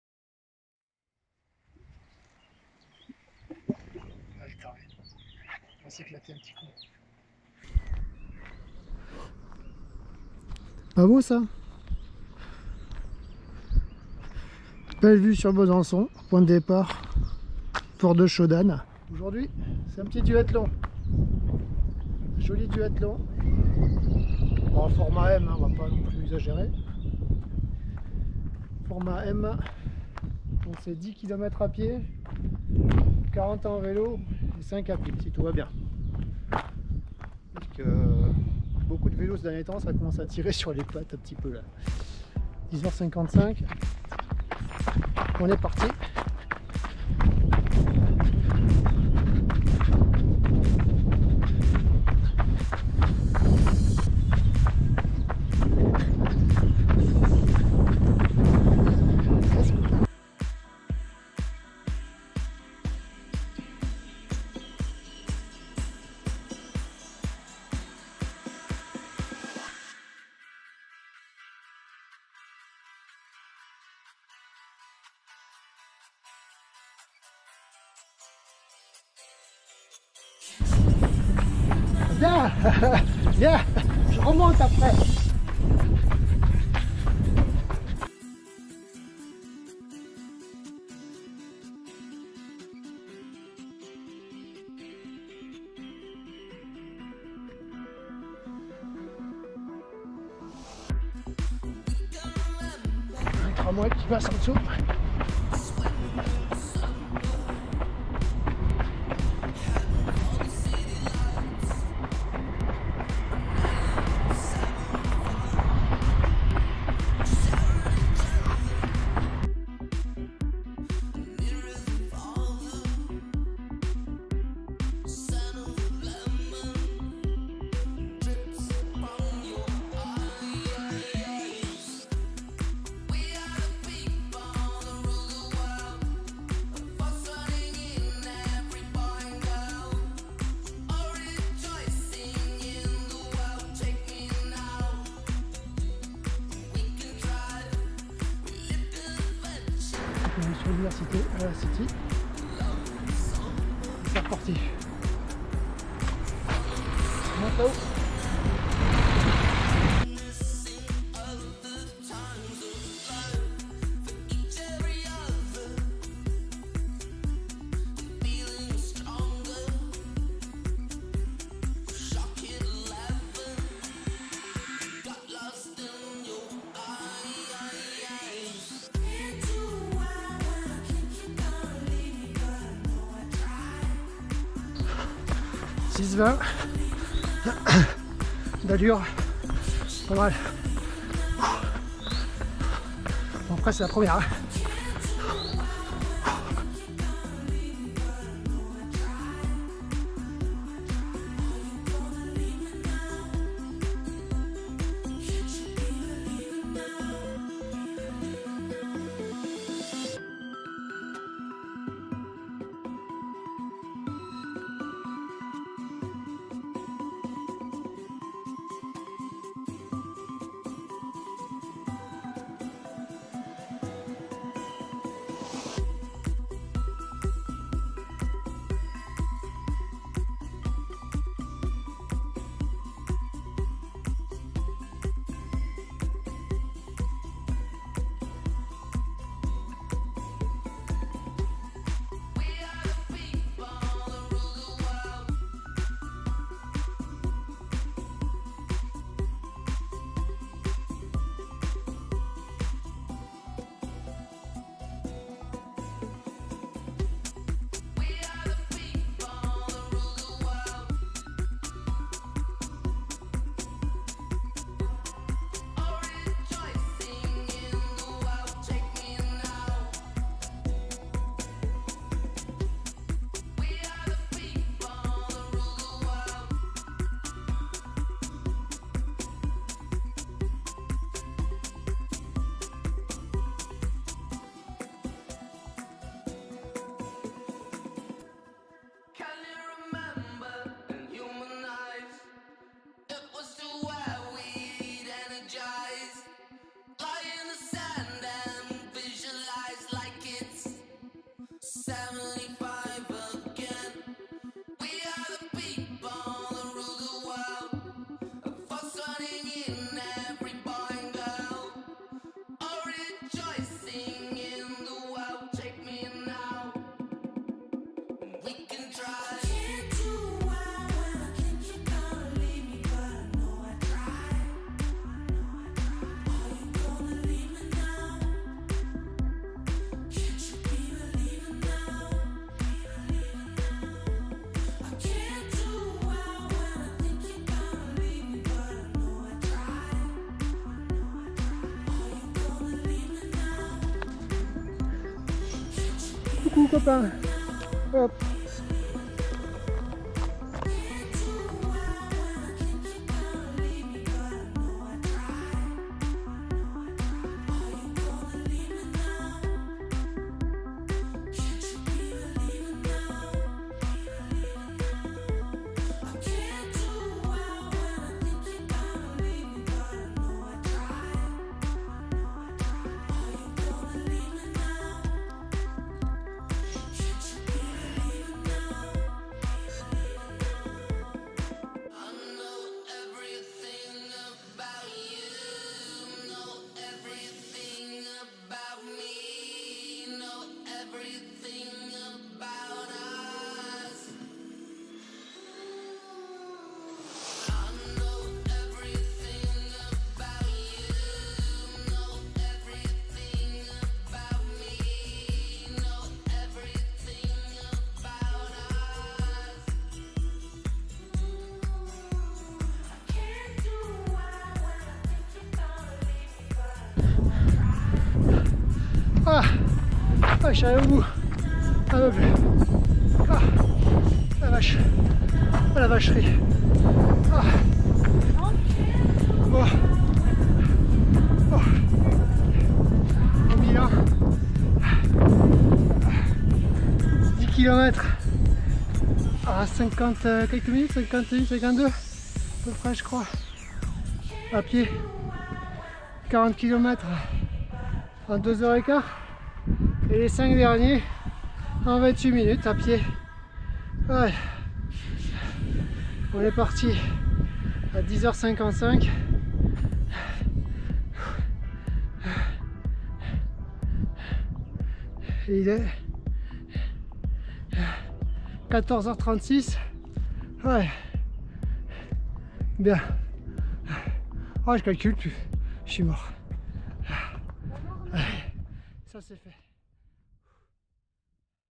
Duathlon la montée de Jean de Gribaldy
Quand la connerie te vient et que tu claques un duathlon sur la montée de Jean de Gribaldy. 10 km de course à pied avec 328 de D+, 40 km en vélo avec 1311 de D+  et pour finir 5 km avec 164 de D+.